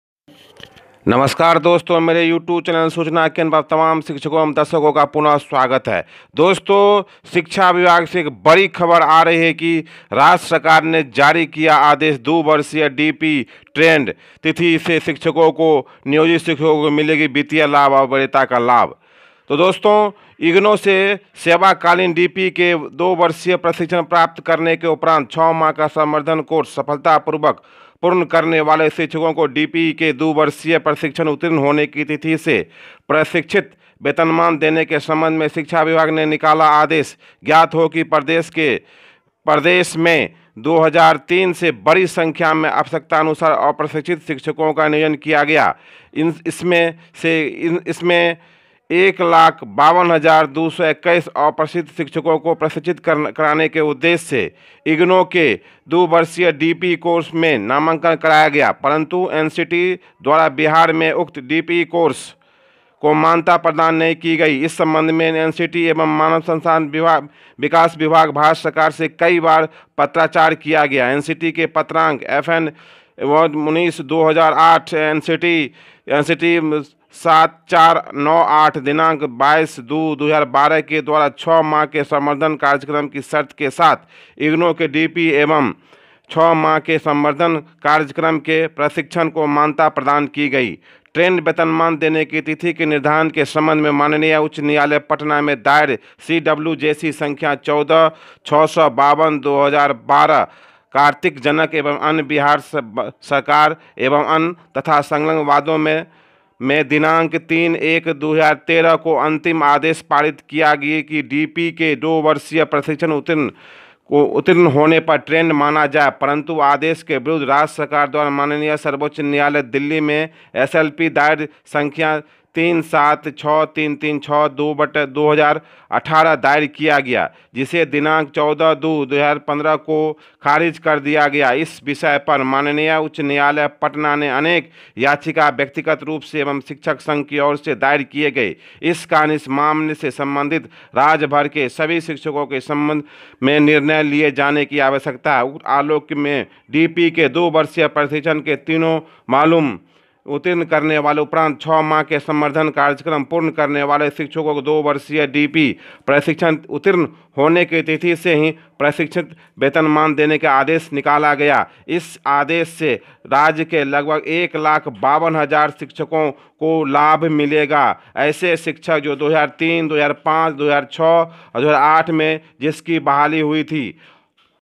खुशखबरी सरकार ने निकाला आदेश DPE प्रशिक्षण प्राप्त शिक्षको को मिलेगा प्रशिक्षित वेतनमान देखें---
SUCHNA _KENDRA